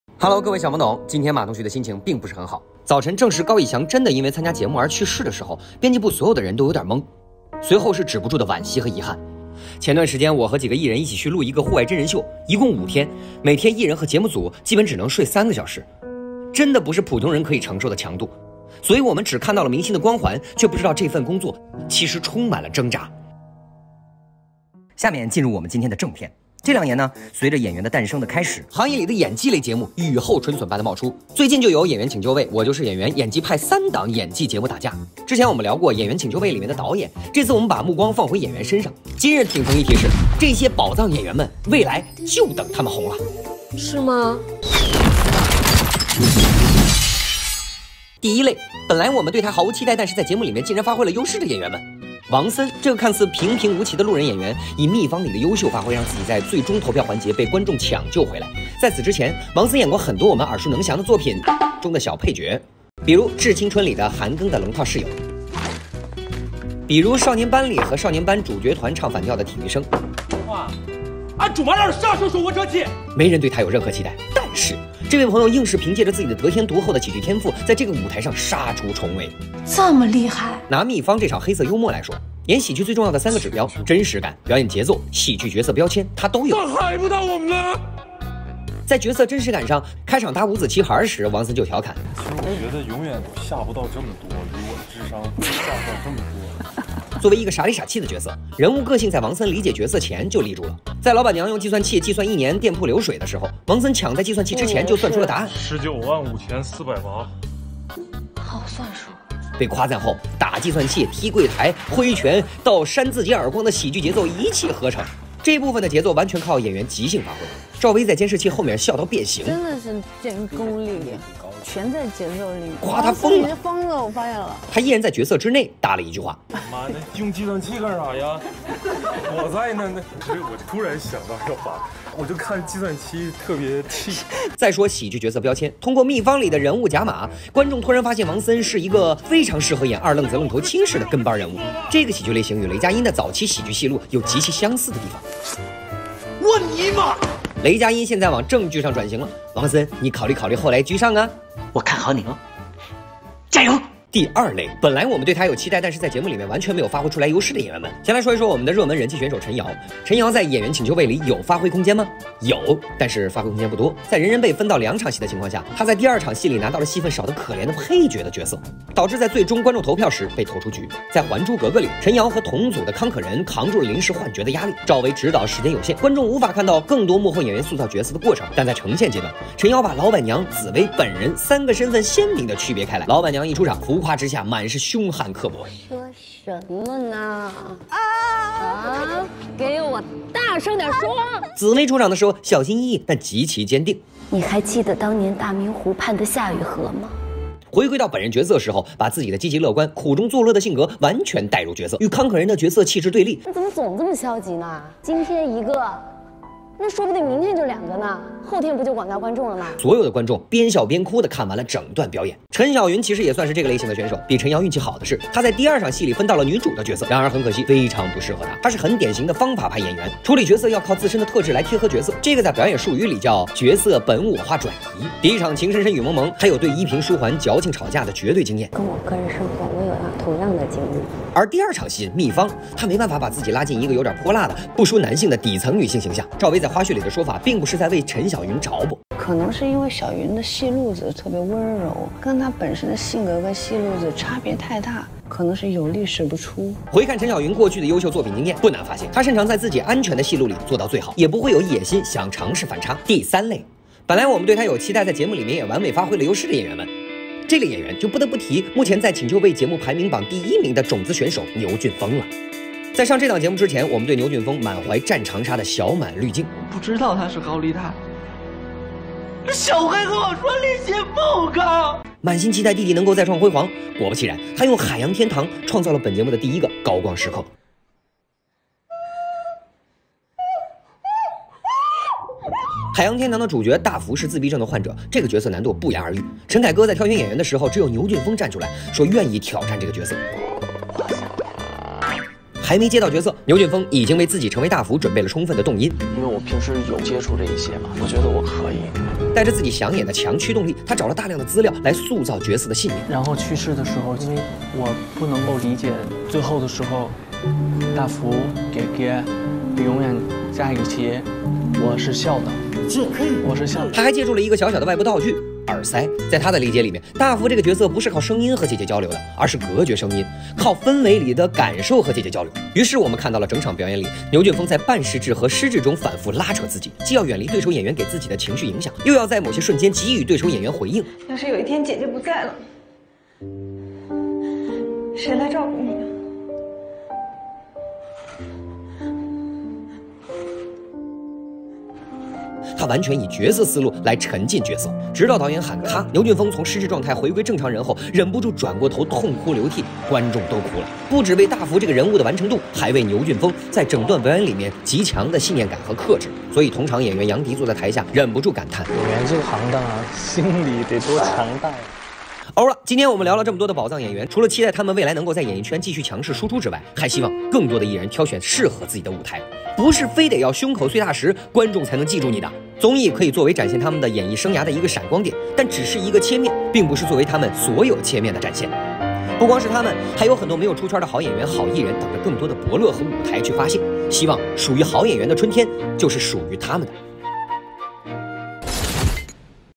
【理娱打挺疼】陈瑶牛骏峰王森，未来就等你们红
他是长在赵薇笑点上人，她是可女人可凶悍可自由的演员，他是陈凯歌认为前途无量的人@王森、@陈瑶、@牛骏峰，未来就等他们红了！